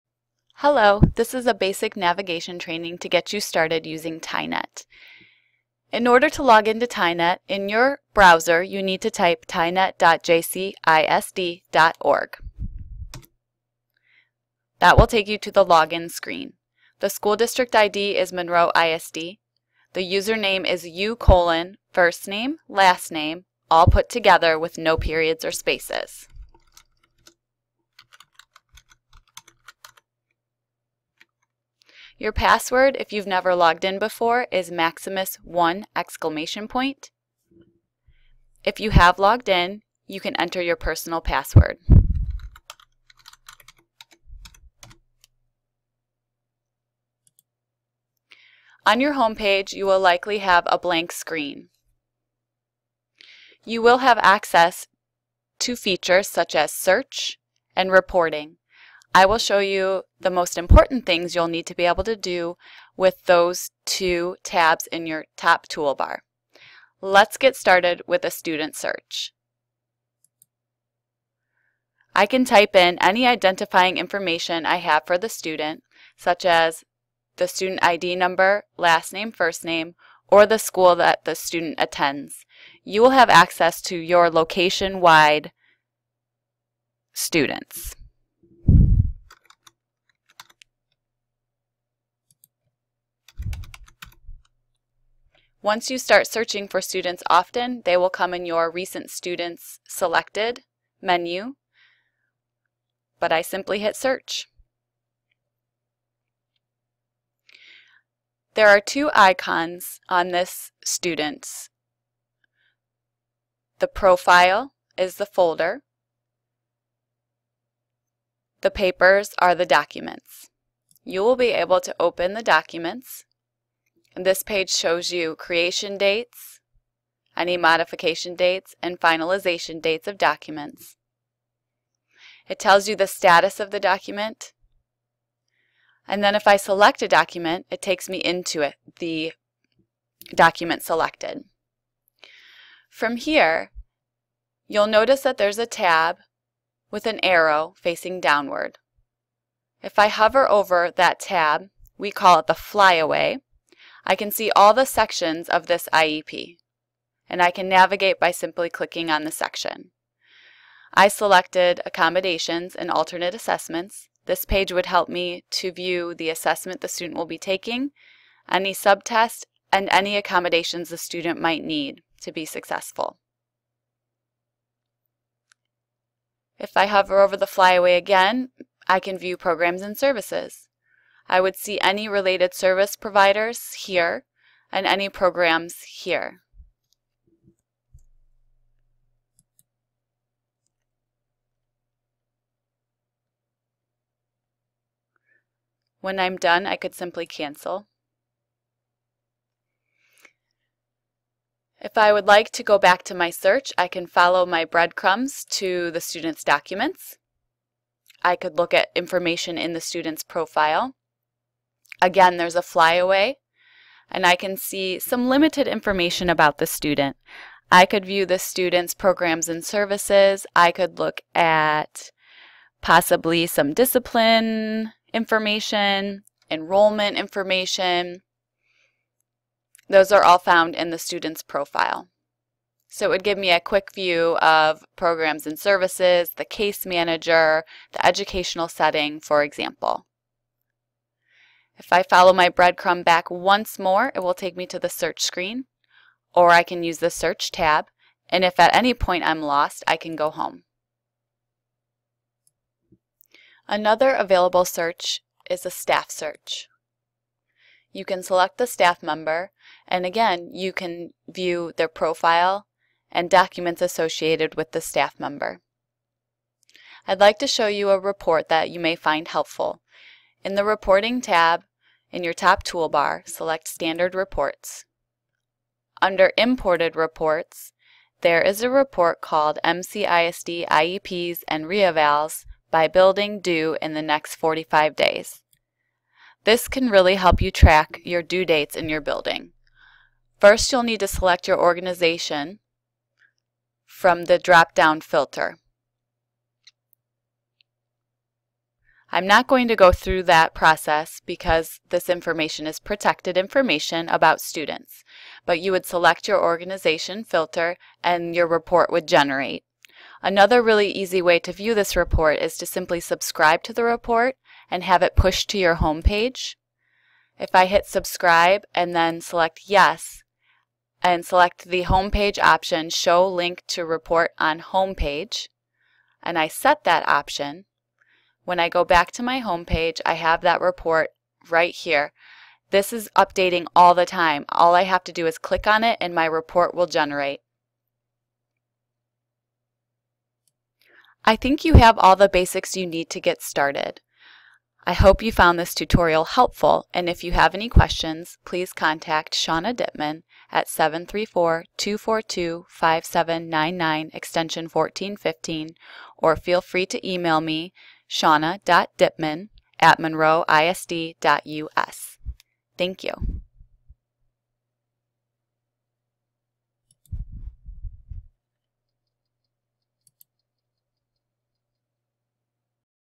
Principal Training-Basic TIENET Navigation
Learn how to search students and staff, view documents and profile information for both.